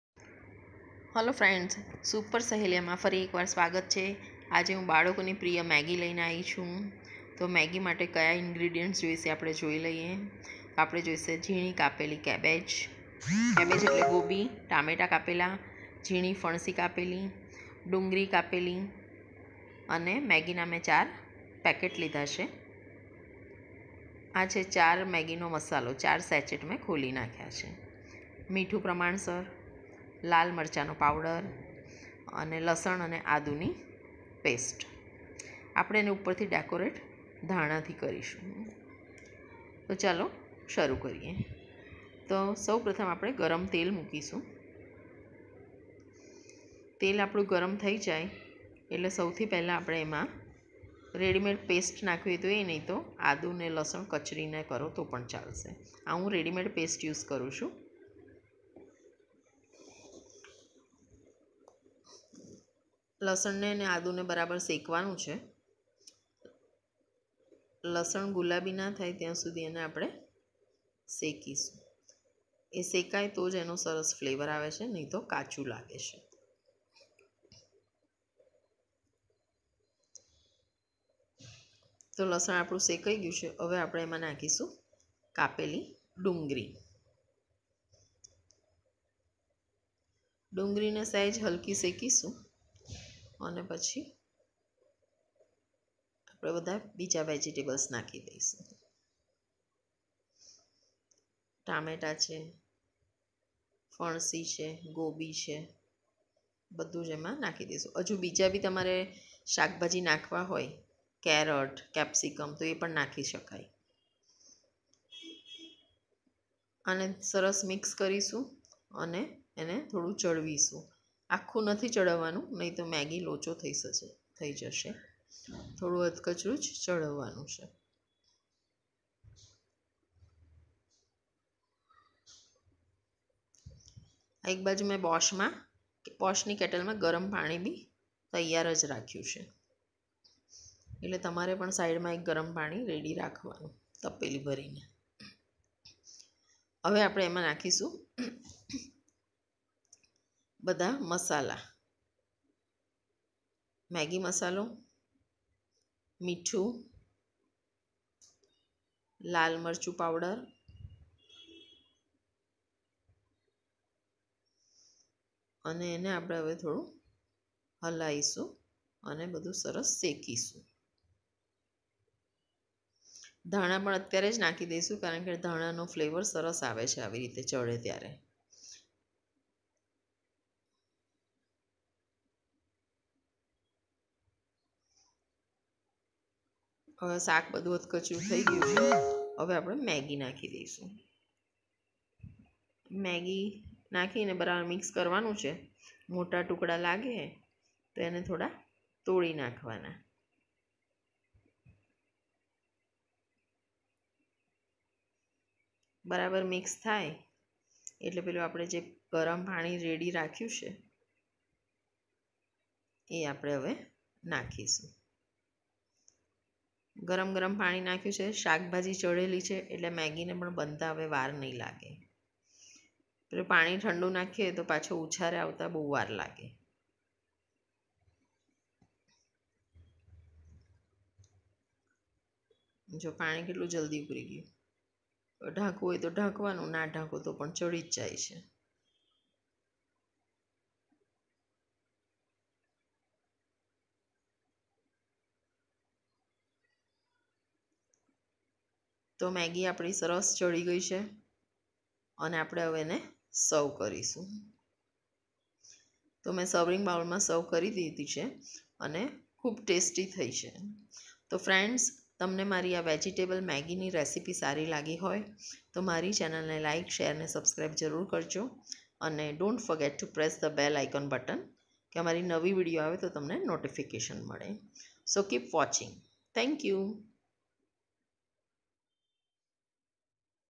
બાળકો ની પ્રિય મેગી પોષ્ટિકતા થી ભરપુર.
પોષ્ટિક મેગી બનાવવા જોઈશે.
મેગી,ડુંગરી, ટામેટા, ફણસી, ગોબી, ગાજર,ધાણા લીલા, મીઠું, મરચું ન મેગી મસાલો,ગરમ પાણી.
વીડિયો મા બતાવ્યા પ્રમાણે બનાવો.